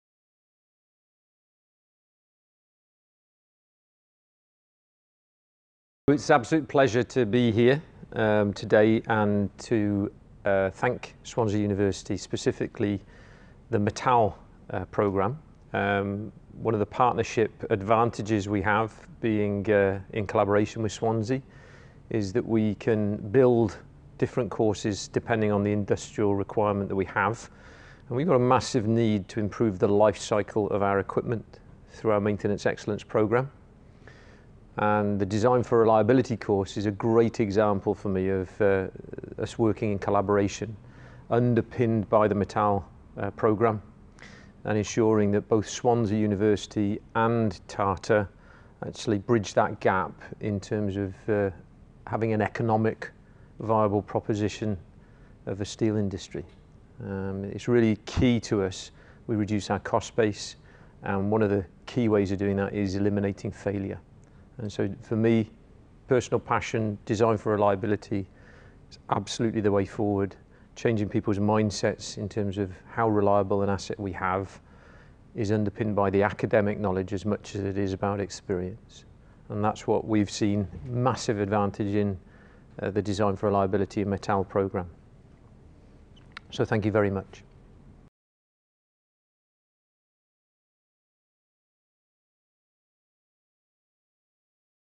Case Study: Design for Reliability - David Bright (Technical Groups Manager, Tata Steel)
David Bright, Technical Groups Manager, Tata Steel.

At TATA’s engineering achievement ceremony on the 25th Jan 2017 David Bright spoke of the importance of our collaboration with Tata and how our newest course, Design for Reliability, course can have a positive impact on their business.

metal delivers high quality materials and manufacturing work-based learning and is funded by the EU’s European Social Fund, through the Welsh Government.